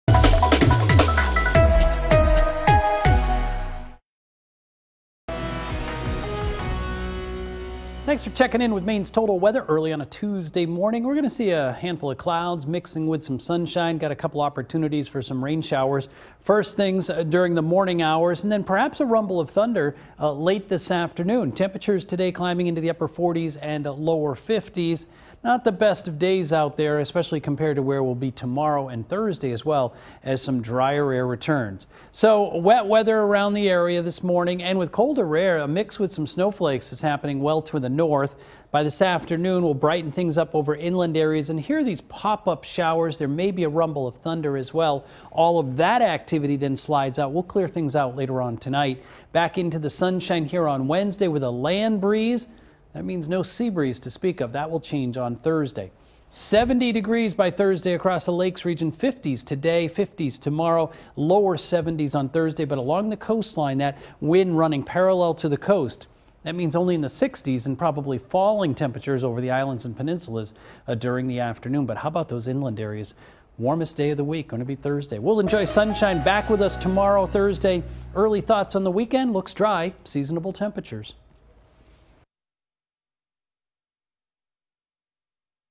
Roger’s Tuesday morning forecast
How's the weather looking for your Tuesday? Get your latest Maine's Total Weather video forecast with Meteorologist Roger Griswold.